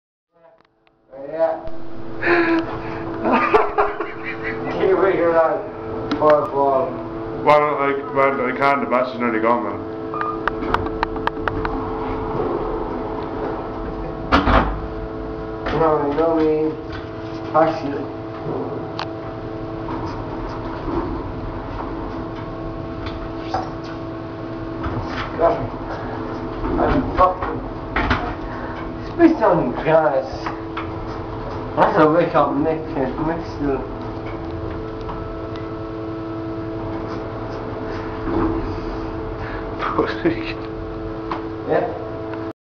Prior vs Elevator
After a long nite at Universal Studios CityWalk Mardi Gras in Orlando FL. the task of gaining stability proved somewhat challenging for Perrys finest